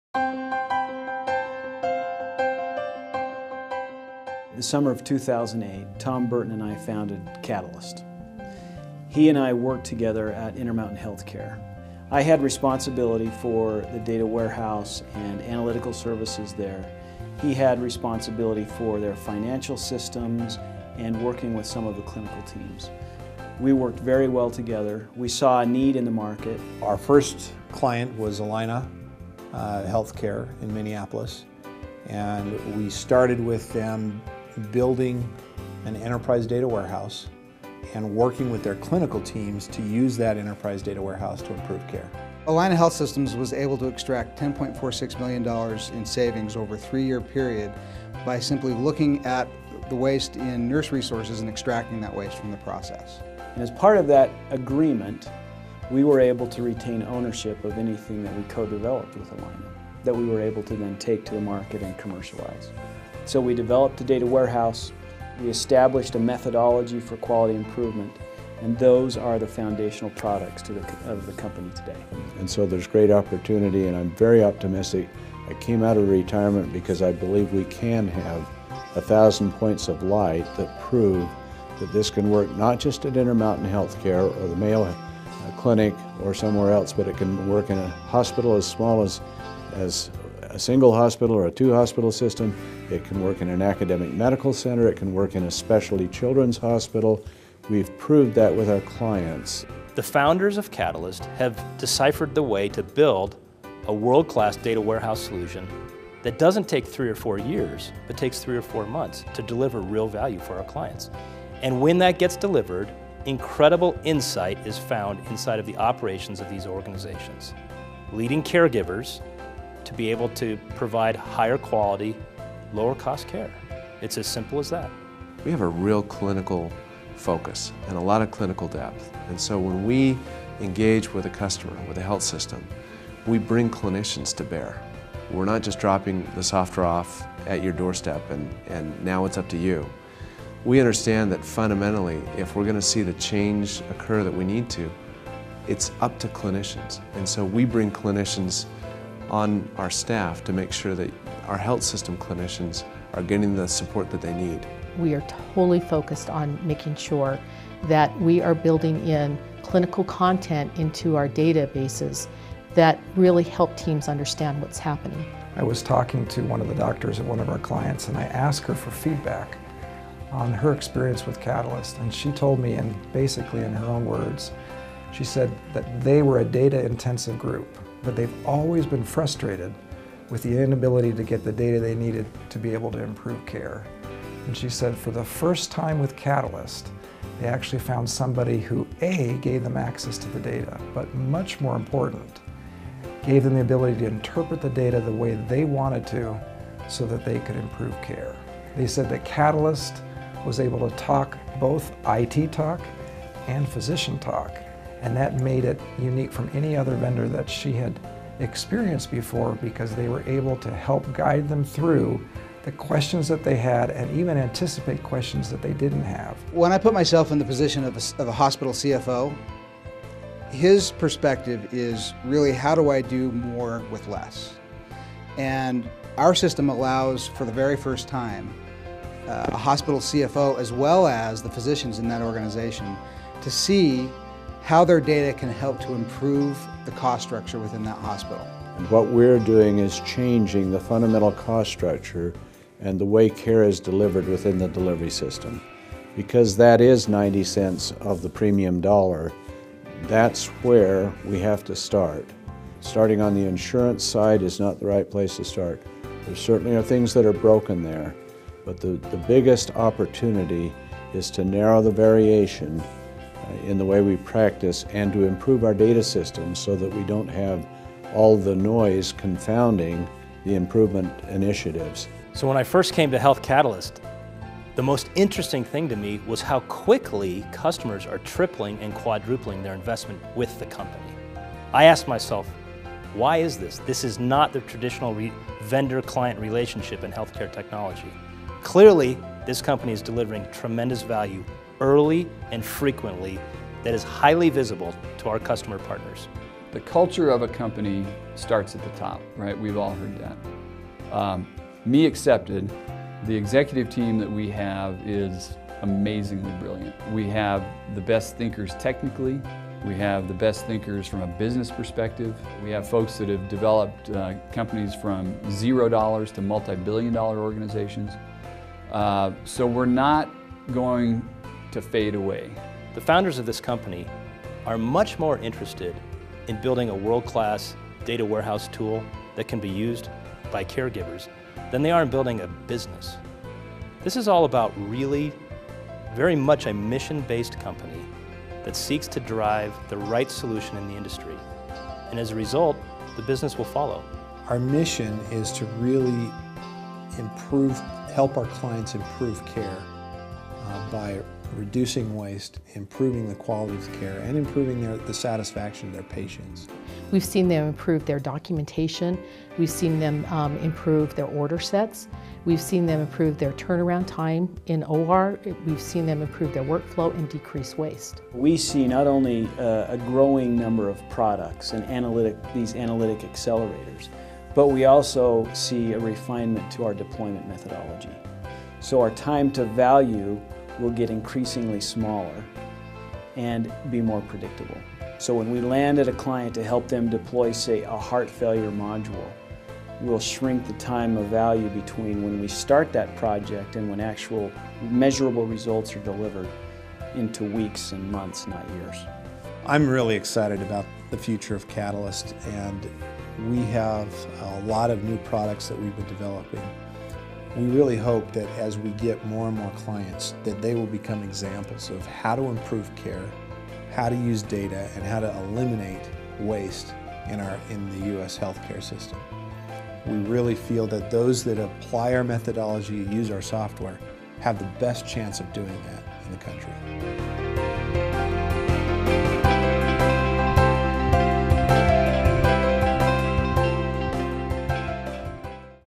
Health Catalyst Overview
Health Catalyst executive leadership shares how the company was formed and focused on helping health systems to improve care and reduce cost by using data warehousing and clinical improvement methodologies.